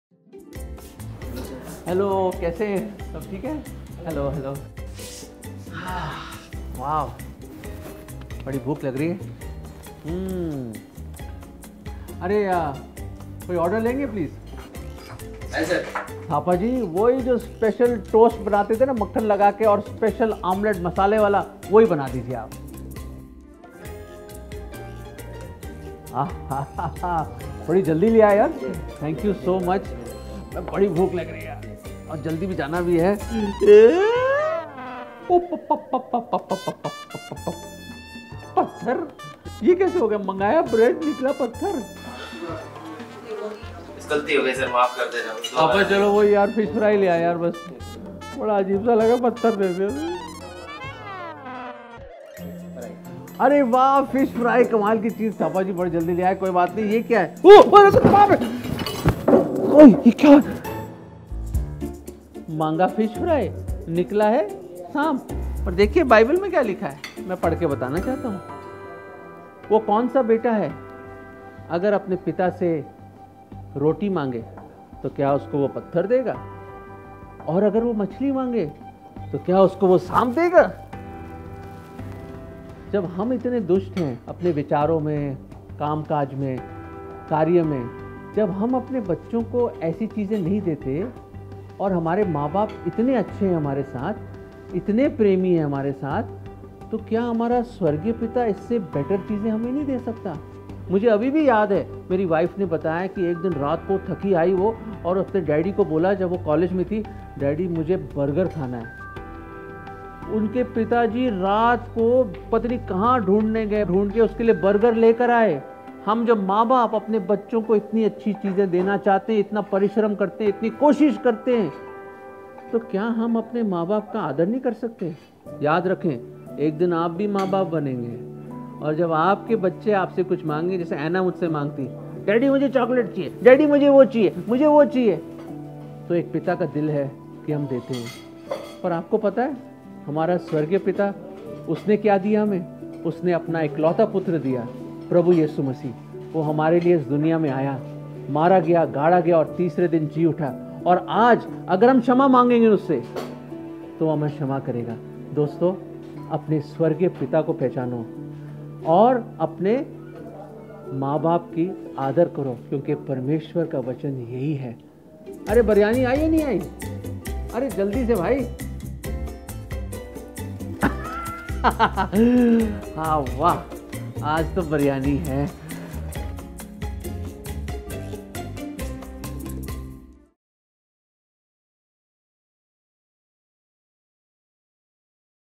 THE FATHER IN HEAVEN GIVES US GOOD THINGS!
If ye then, being evil, know how to give good gifts unto your children, how much more shall your Father which is in heaven give good things to them that ask him? Matthew 7:11